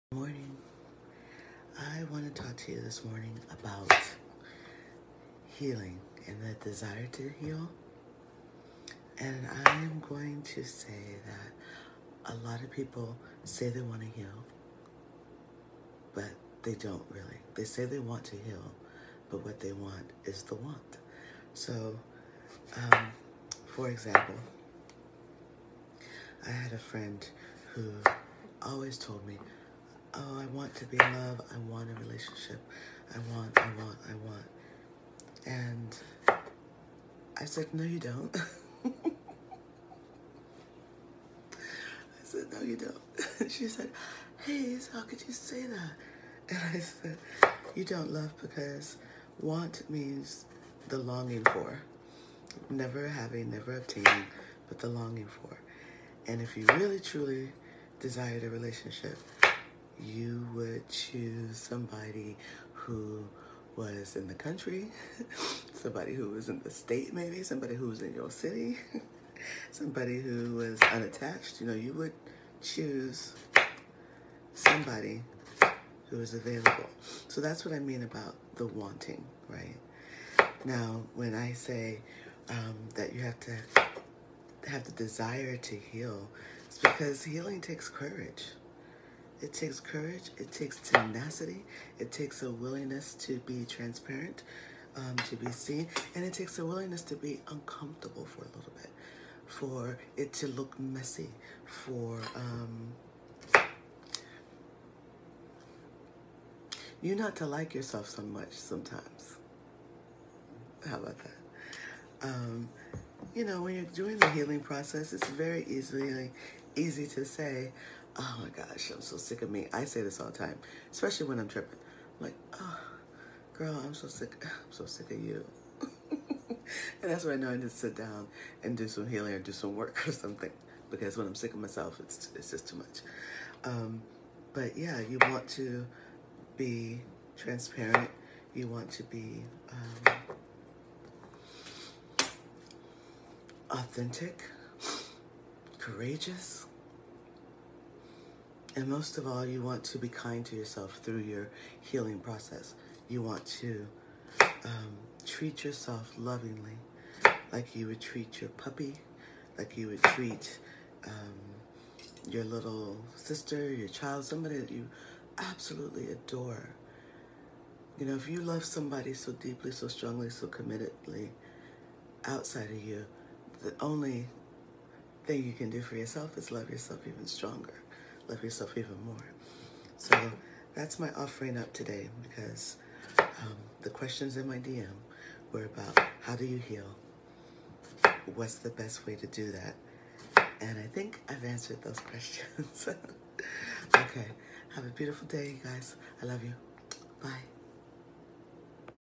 So You Want to Heal
The tools you need and the attitude you need to do your personal healing work. Be clear on your desire to heal.

✨ Follow Haize ✨